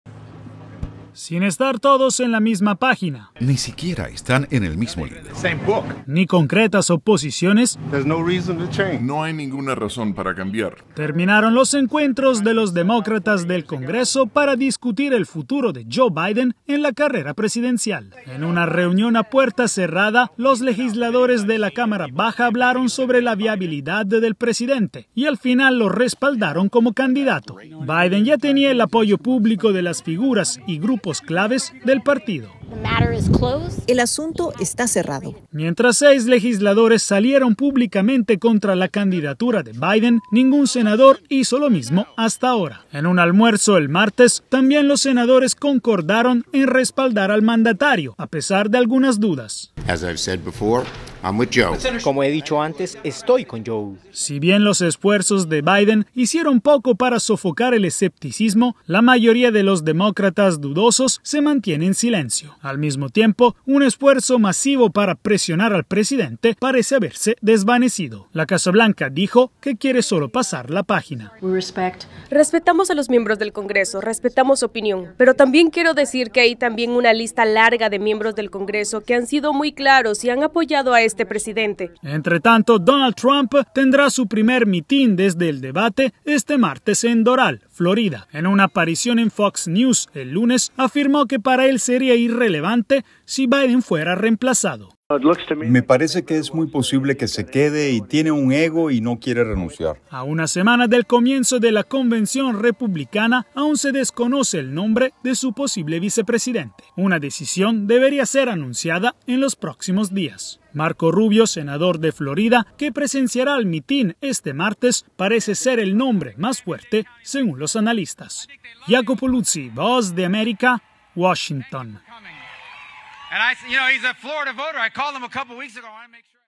ELECCIONES Demócratas se unen para apoyar a Biden pese a las dudas
Descripción: El presidente Joe Biden enfrentó reuniones de demócratas en el Congreso cruciales para medir el respaldo de su propio partido a su candidatura. No existe un consenso unánime entre los legisladores, pero el apoyo al presidente quedó garantizado. Iacopo Luzi, de la Voz de América, informa.